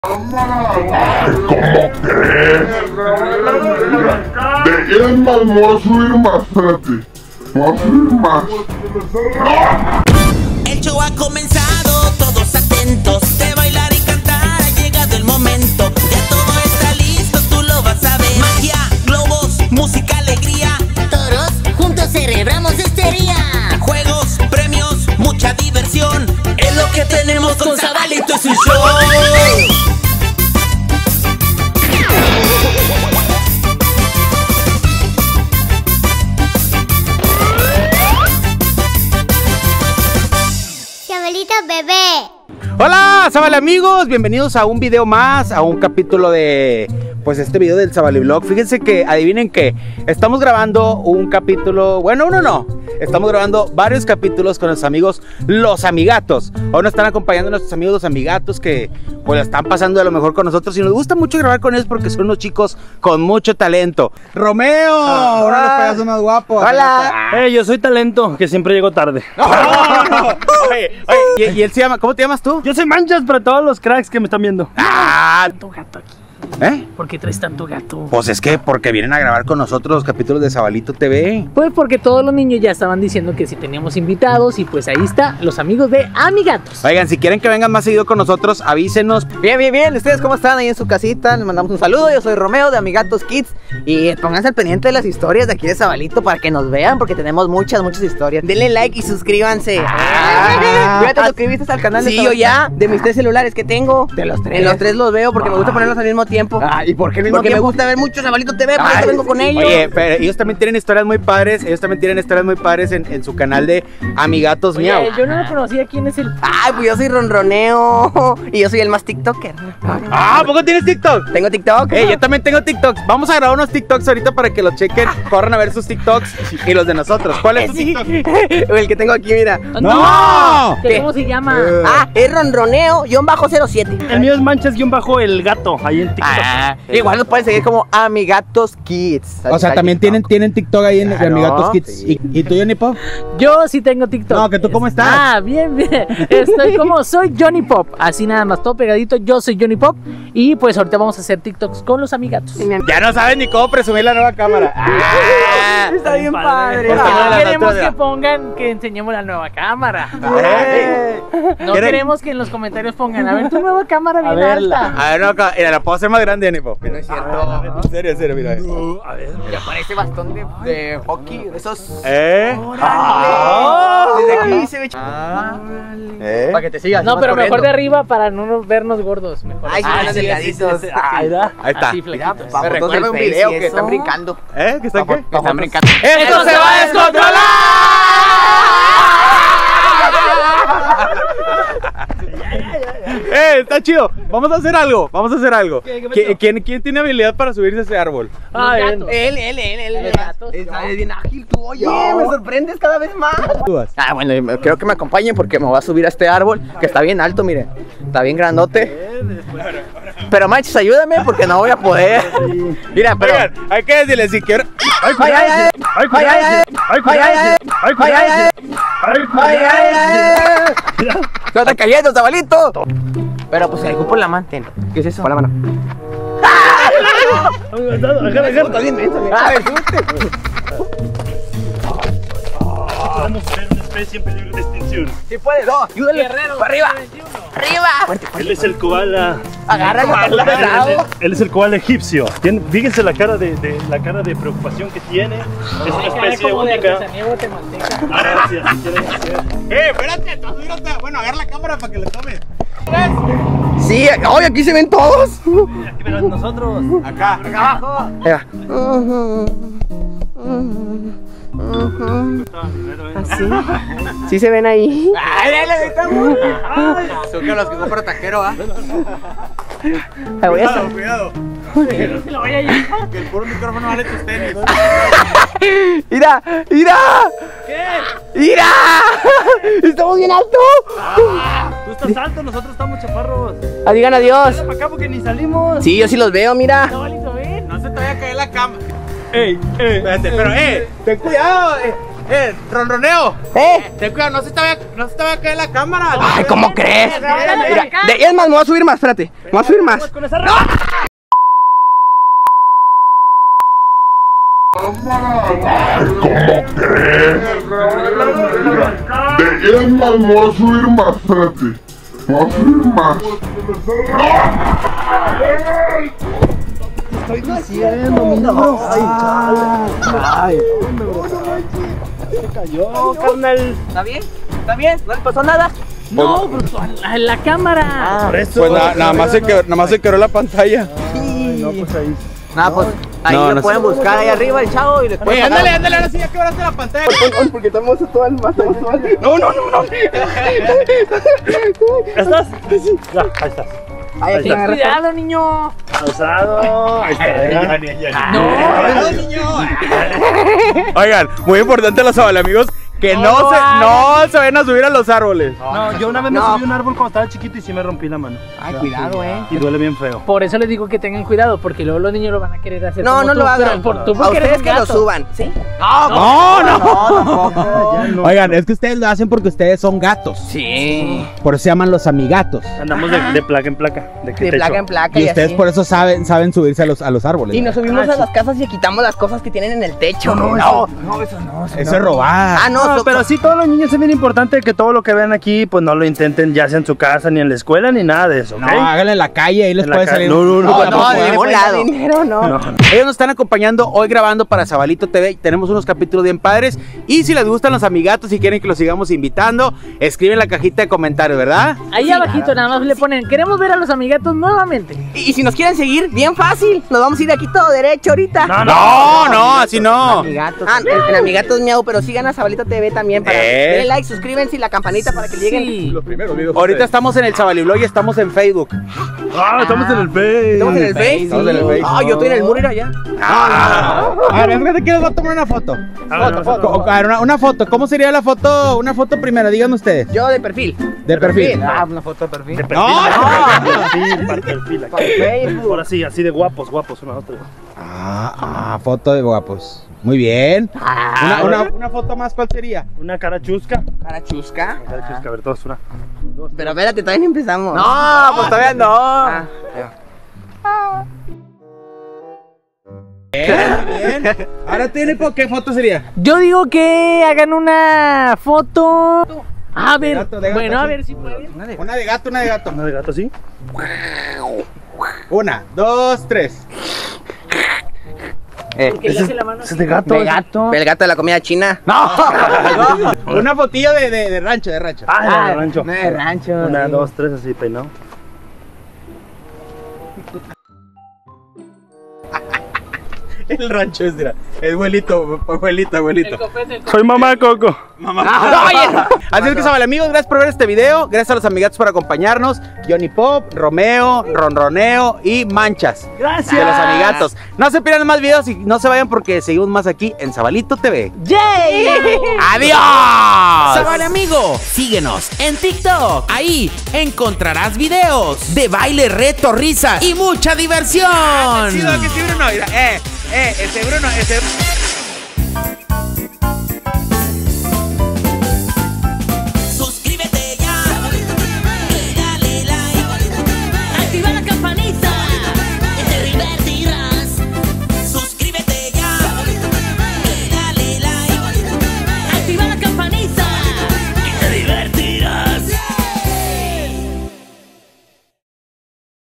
GATO Atrapado en un ARBOL | Zabalito Tv
Hola AMIGOS estuvimos en un Parque y un Amigato se quedo atrapado

Contrataciones En Toda La Republica Mexicana
Monterrey, N.L, Mexico.